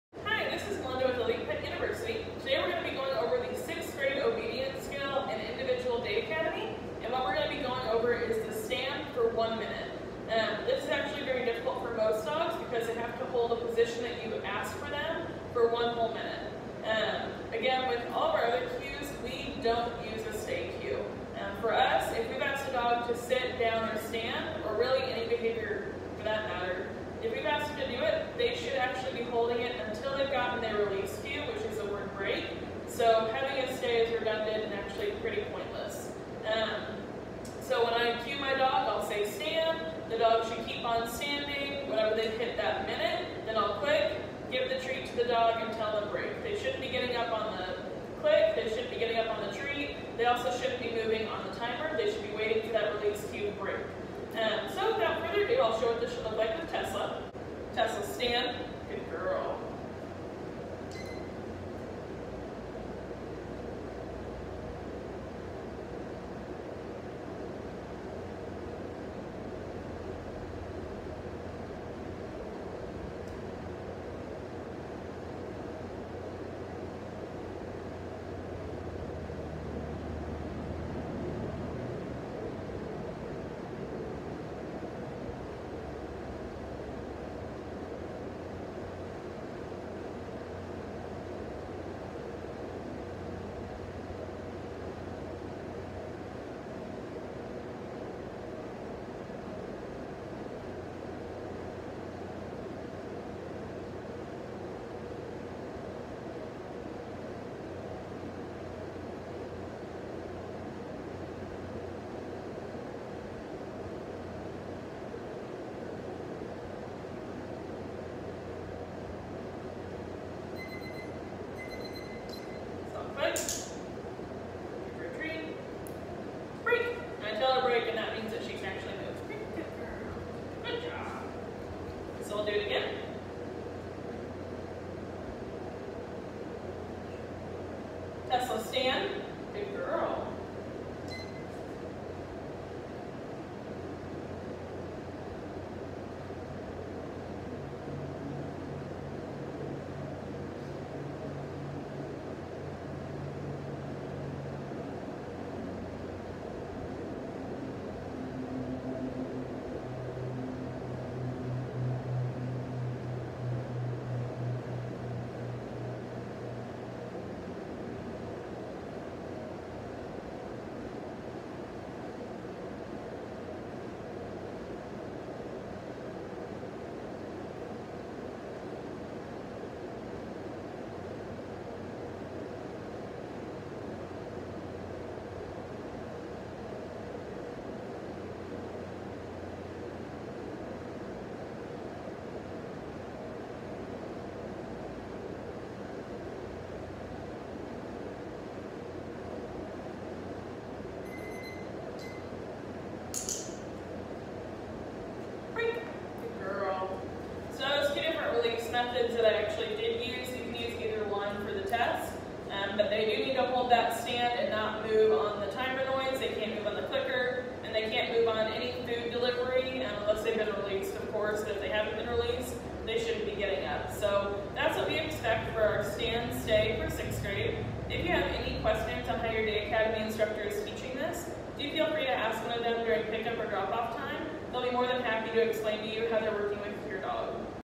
Day Academy 6th Grade Stand
In 6th grade of Day Academy, we start teaching our students the beginning of "stay" work, that is, maintaining the cue they were asked for a certain amount of duration. In 6th grade, we require the dog to hold the position they were cued for one minute until they hear their release cue break. In this video, this is an example of what we would expect a 6th grade Day Academy student to be able to pass for stand duration on test day.